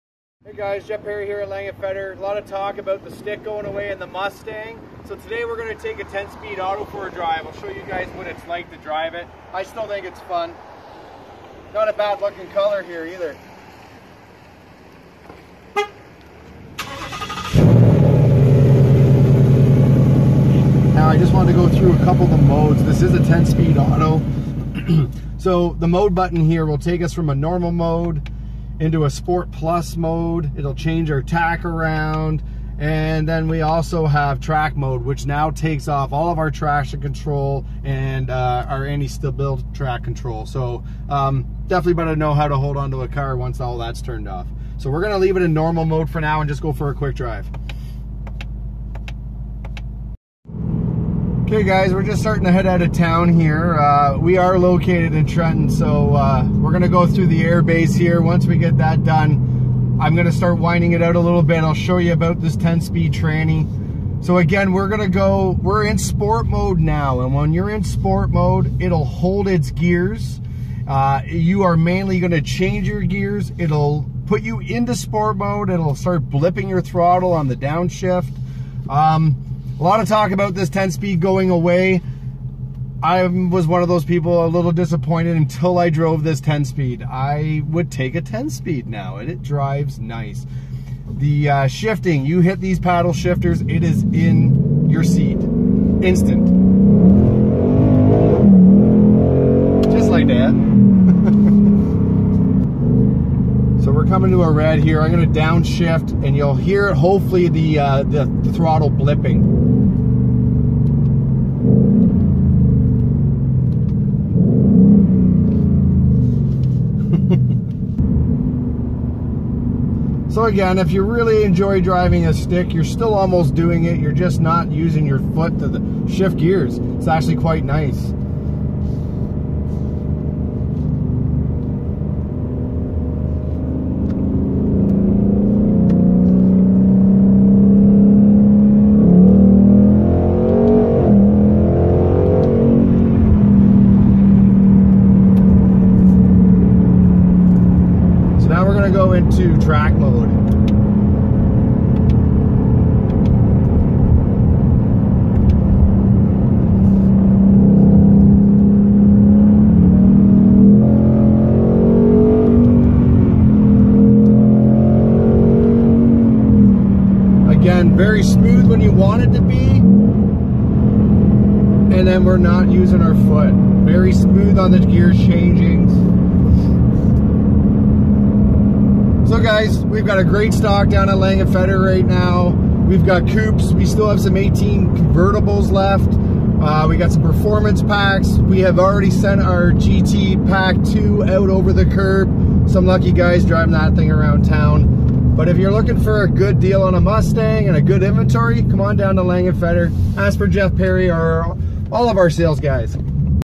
10 speed Auto Mustang review
10 Speed automatic and what I think vs a manual. It is a great working car 2019 Mustang GT premium Pkg with performance pkg. @langefetterford @parry33_fordtruckguy